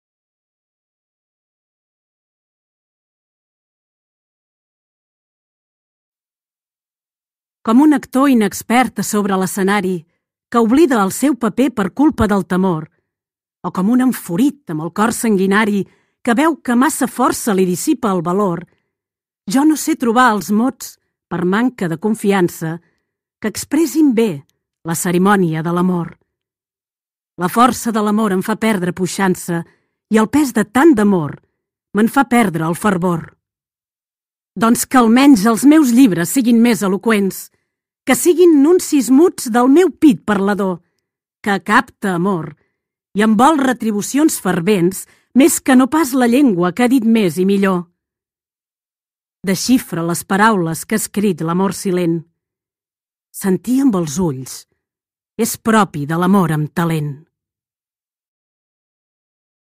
Shakespeare Sonet 23 VEU I TEXT
El col·lectiu En Veu Alta posa veu, en català  (versió de Salvador Oliva),  al sonet número 23 de William Shakespeare,  La veu s'acompanya amb el text del sonet.